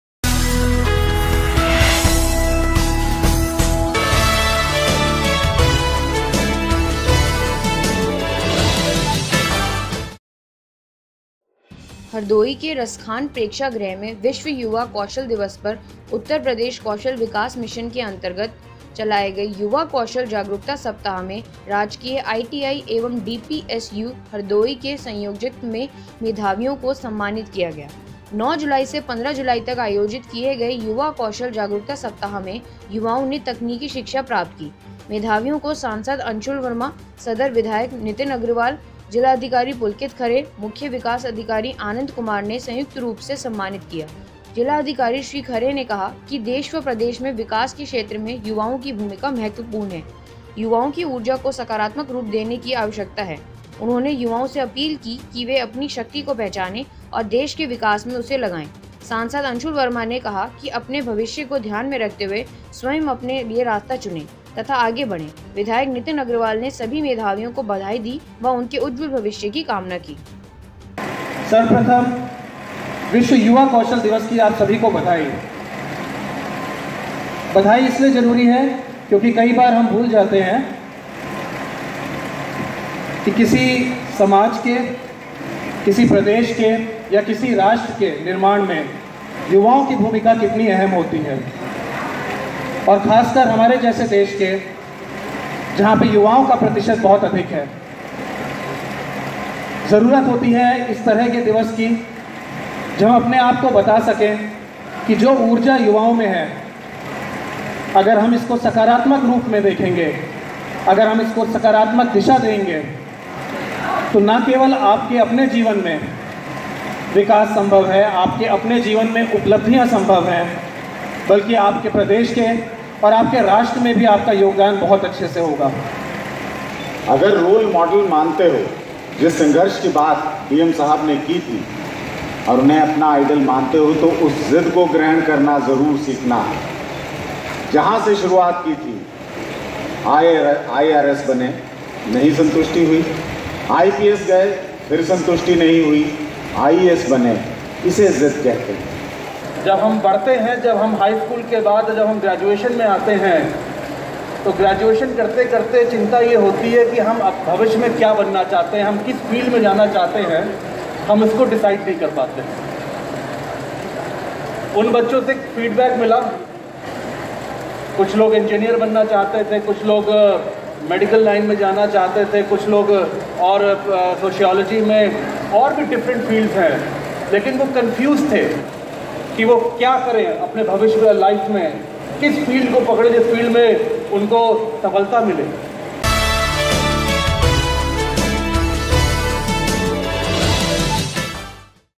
विश्व युवा कौशल दिवस पर रसखान प्रेक्षागृह में सम्मानित किए गए मेधावी-Today24
हरदोई के रसखान प्रेक्षागृह में विश्व युवा कौशल दिवस पर उत्तर प्रदेश कौशल विकास मिशन के अंतर्गत चलाए गए युवा कौशल जागरूकता सप्ताह में राजकीय आईटीआई एवं डीपीएम यू हरदोई के संयोजकत्व में मेधावियों को सम्मानित किया गया 9 जुलाई से 15 जुलाई तक आयोजित किए गए युवा कौशल जागरूकता सप्ताह में युवाओं ने तकनीकी शिक्षा प्राप्त की मेधावियों को सांसद अंशुल वर्मा सदर विधायक नितिन अग्रवाल जिलाधिकारी पुलकित खरे मुख्य विकास अधिकारी आनंद कुमार ने संयुक्त रूप से सम्मानित किया जिलाधिकारी श्री खरे ने कहा कि देश व प्रदेश में विकास के क्षेत्र में युवाओं की भूमिका महत्वपूर्ण है युवाओं की उर्जा को सकारात्मक रूप देने की आवश्यकता है उन्होंने युवाओं से अपील की कि वे अपनी शक्ति को पहचाने और देश के विकास में उसे लगाएं। सांसद अंशुल वर्मा ने कहा की अपने भविष्य को ध्यान में रखते हुए स्वयं अपने लिए रास्ता चुने तथा आगे बढ़े विधायक नितिन अग्रवाल ने सभी मेधावियों को बधाई दी व उनके उज्जवल भविष्य की कामना की।

T.V. News

Application.

views!!!

News."